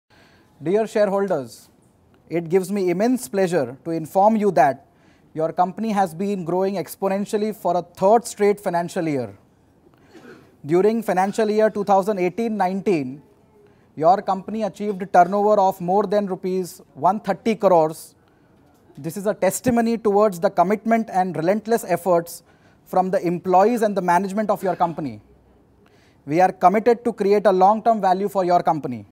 AEL AGM Video
Chairman & Managing Director of Abans Enterprises Limited, Mr. Abhishek Bansal, at its 33rd AGM, shared the following milestones achieved during the year: exponential growth for a 3rd straight financial year, acquisition of Abans Jewels Private Limited.